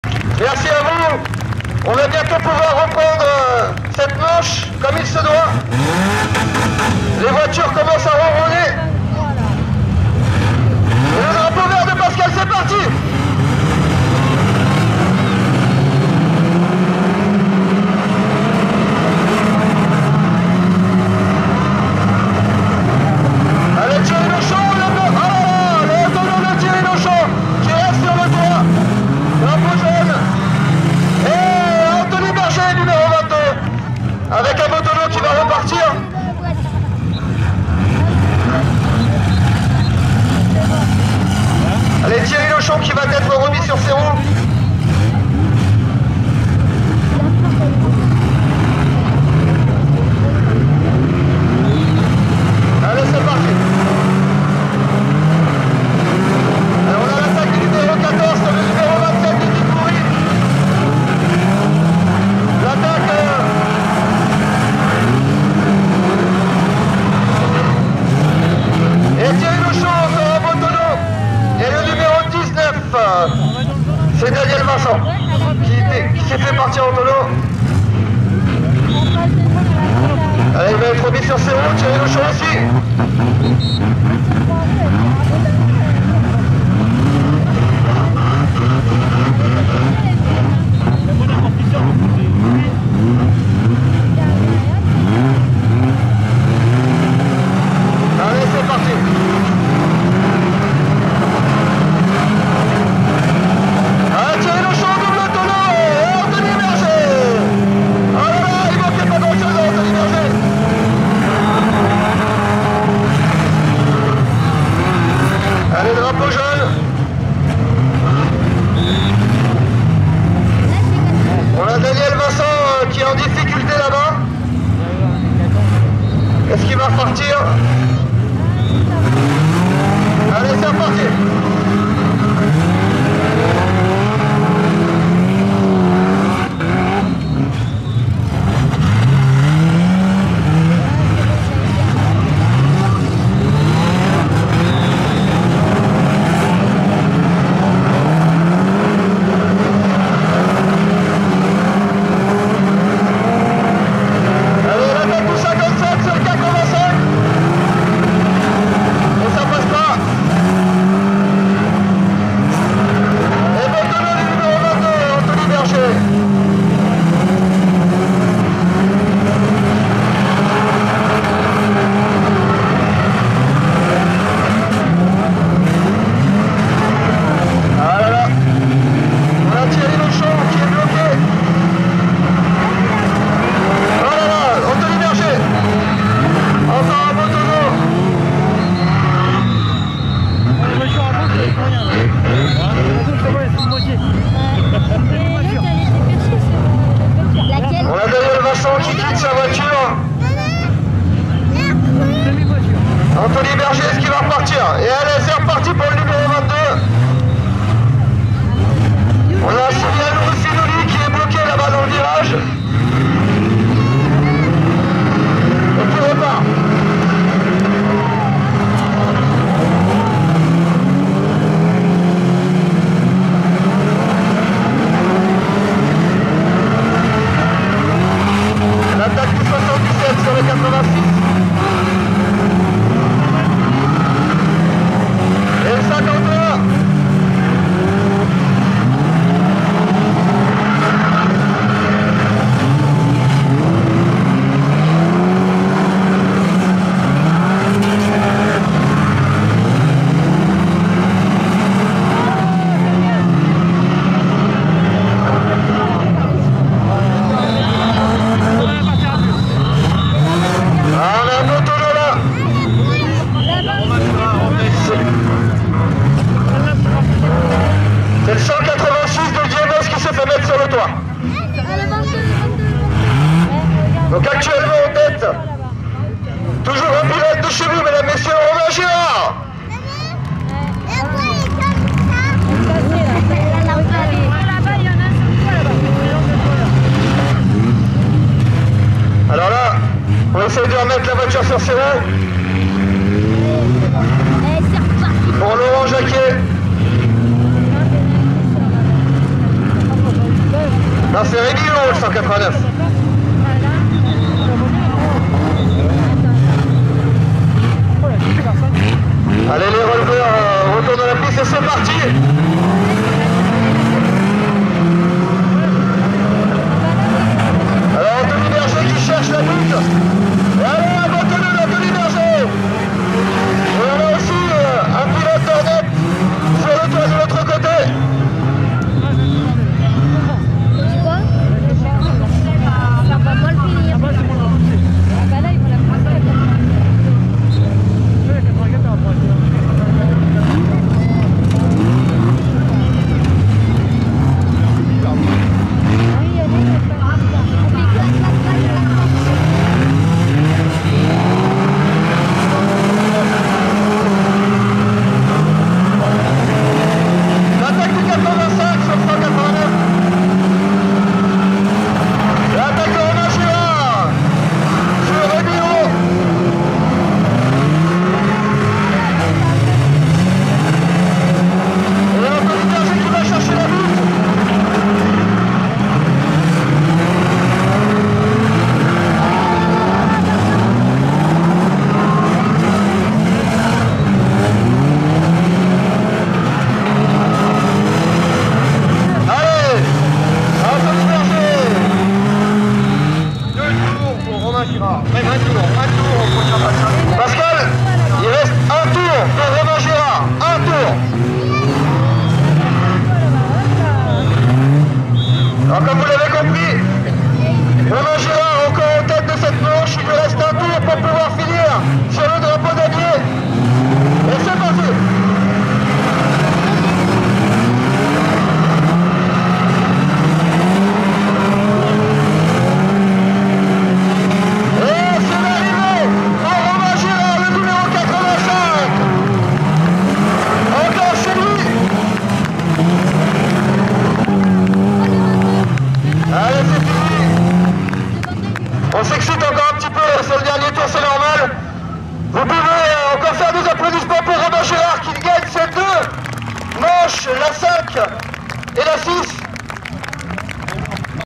Super Stock-car 2020 - Beaumont du Gâtinais - Manche 6 (2ème partie) 4K
Super Stock-car à Beaumont du Gâtinais - La 2ème partie de la Manche 6 après l’interruption au drapeau rouge.

Filmé en 4K